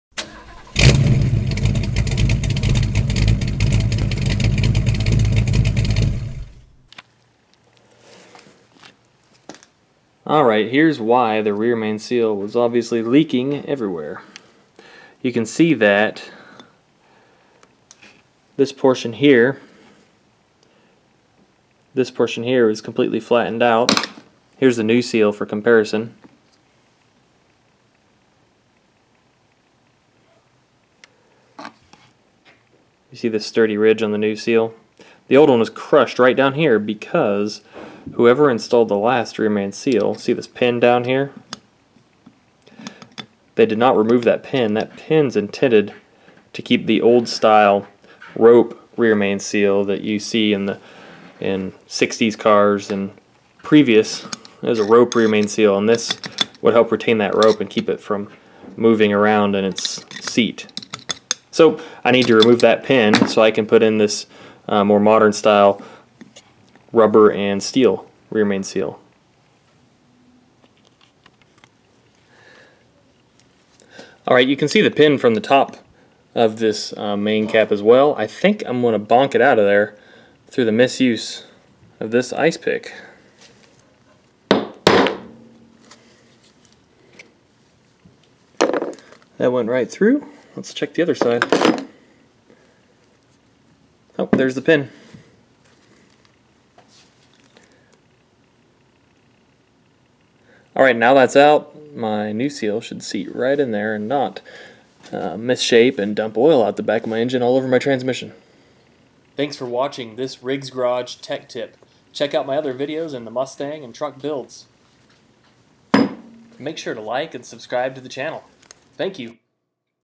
Remove that Rope Seal Pin! Prevent Small Block Ford Rear Main Seal Leaks
2 minute tech tip! Early small block Ford (and many other engines) rear main seals were rope style instead of steel-reinforced rubber. My engine came to me with this rear main leak that was a result of the rope seal pin that was not removed when the new style rubber seal was added in the past.

Here's a quality rear main seal if you need one